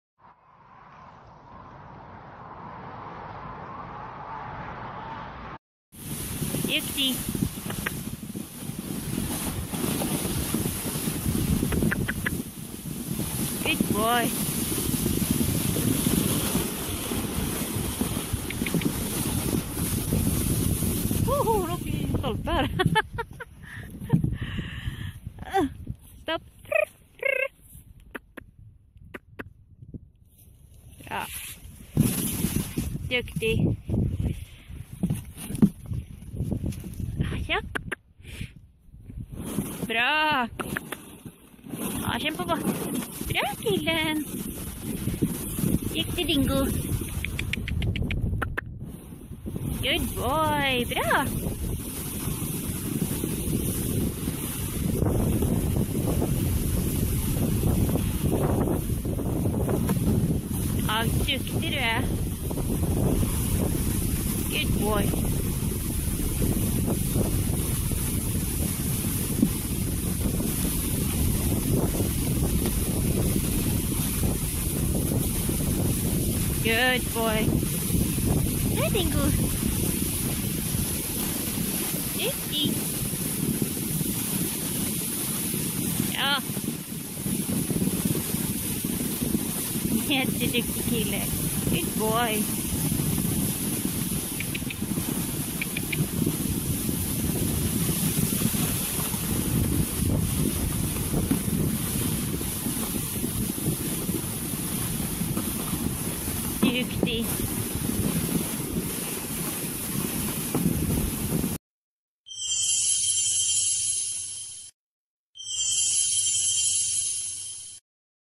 Pulkakörning med Dingo ~ 13/11-16
Jag åker pulka efter Dingo som var jätteduktig! Välkommen till